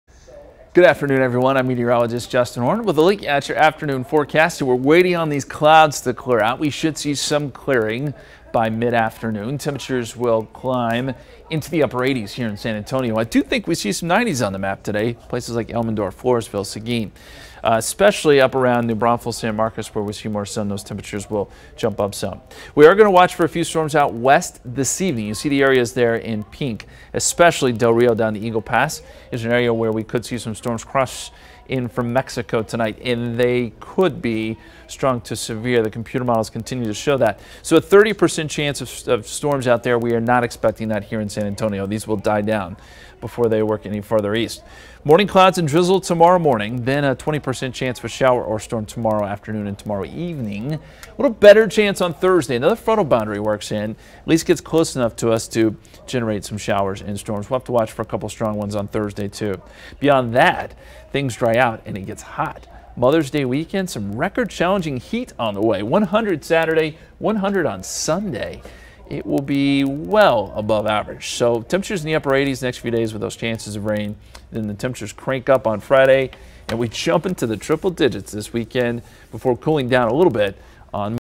Justin's Midday Update: May 3, 2022
Some clearing skies this afternoon, with warm temperatures.